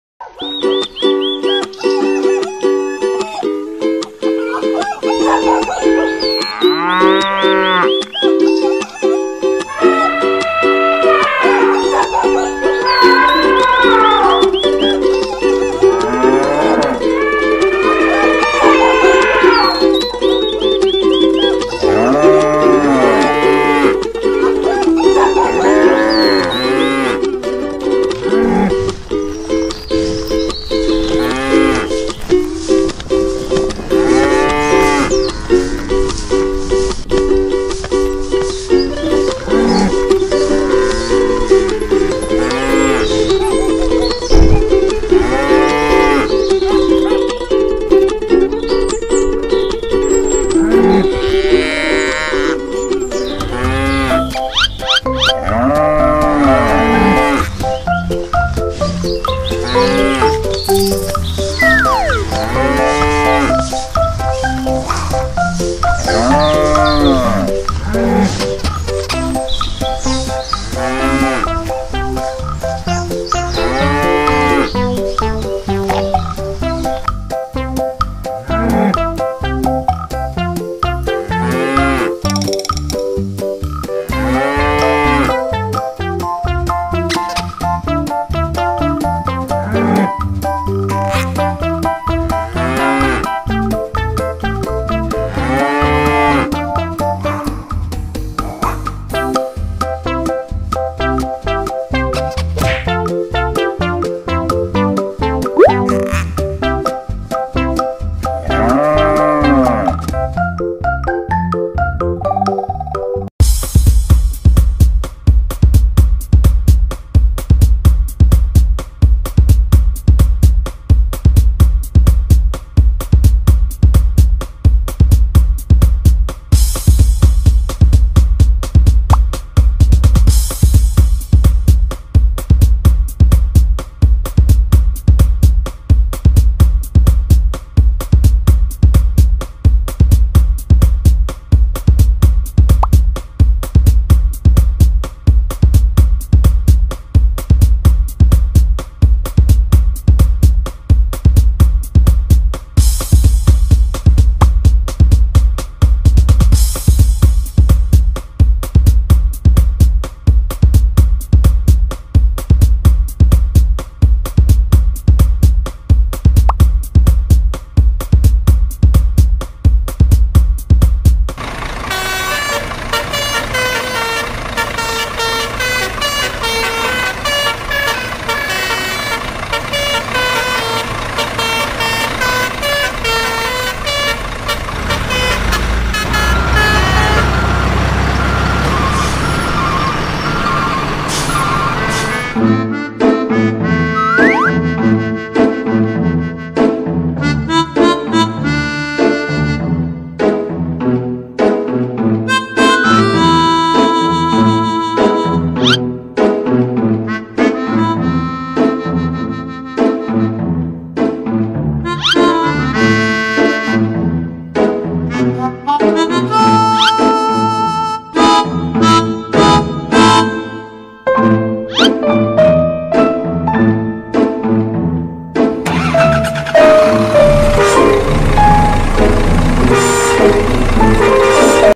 WADIDAU‼️ SIMULASI PROSES POTONG SAPI | EVO TOY
WADIDAU SIMULASI PROSES POTONG SAPI !!
MANUSIA KARDUS

Media Video:

Mainan Sapi
Pewarna makanan
Truk angkut sapi

Video ini hanya ilustrasi bertujuan untuk hiburan